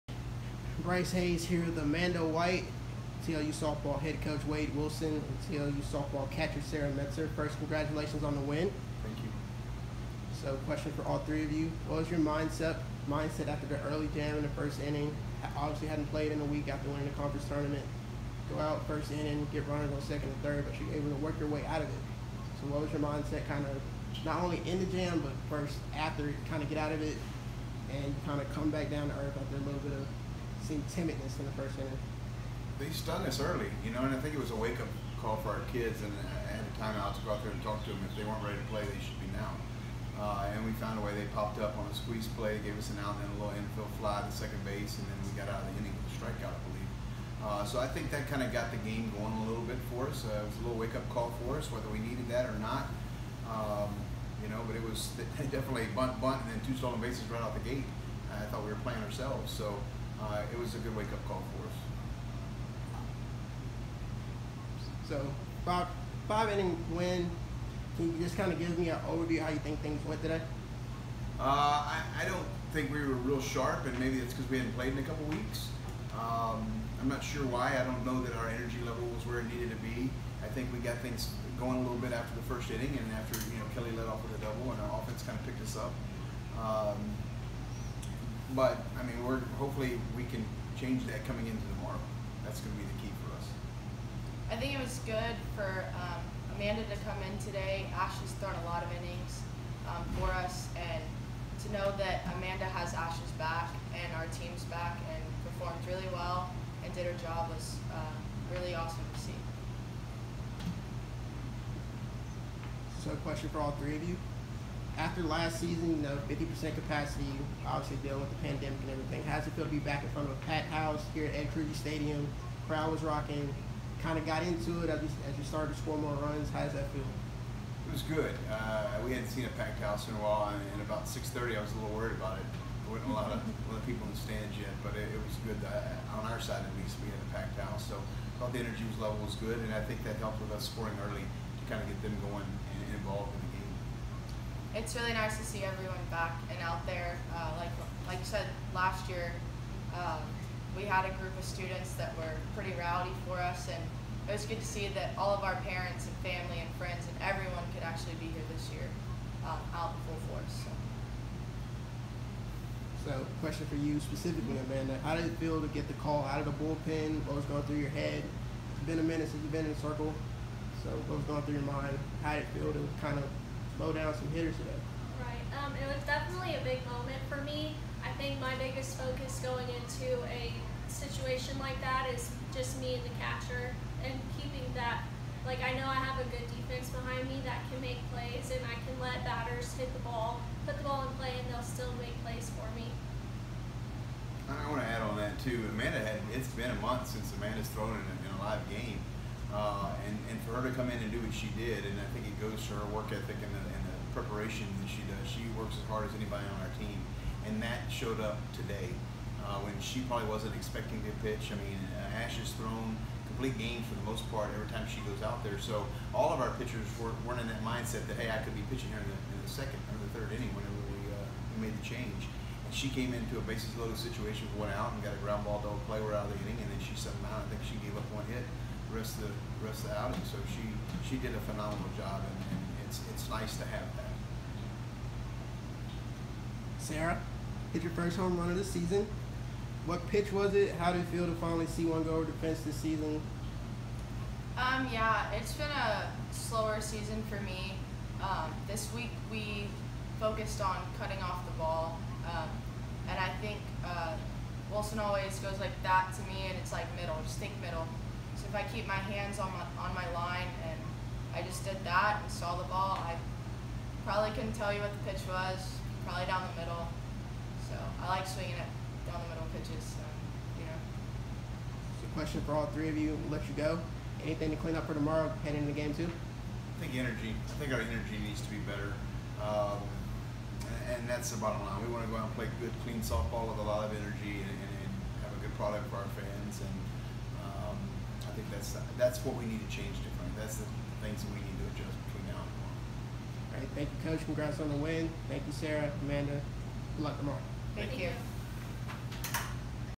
Texas Lutheran Softball Interview After 9-1 Run-Rule Victory over Chapman Univ.
TLU Softball Head Coach Wade Wilson, Pitcher Amanda White, and Catcher Sarah Metzer discuss their win in game one of the 2022 Seguin, Texas Regional